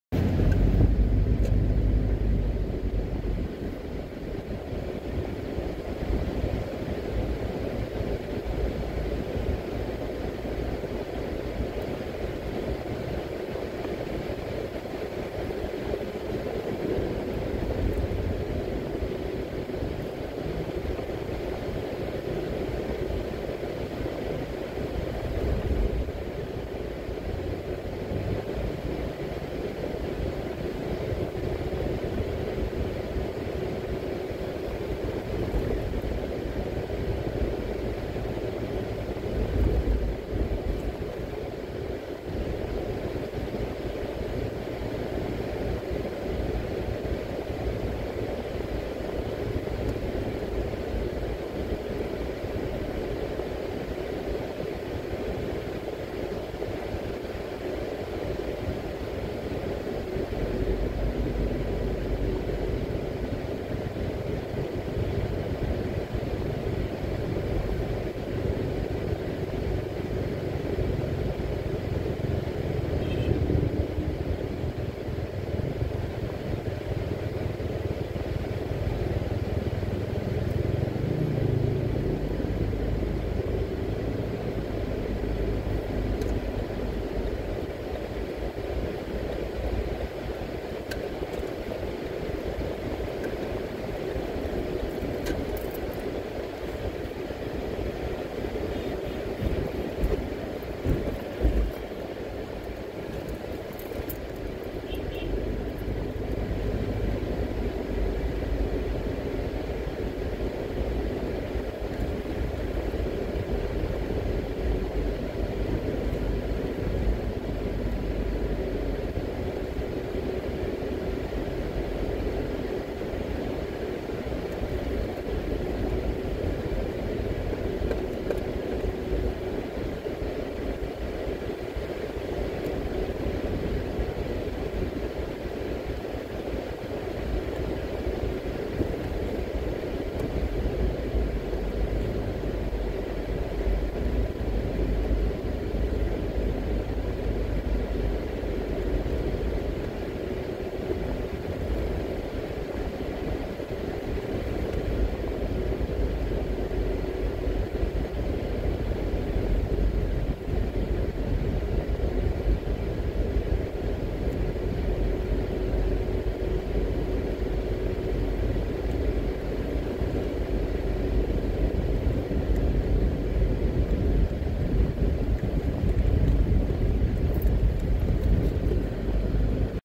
Driving on Barbican Road | Kingston | Jamaica
Driving on Barbican Road in Kingston Jamaica. Driving on Old Hope Road in Kingston Jamaica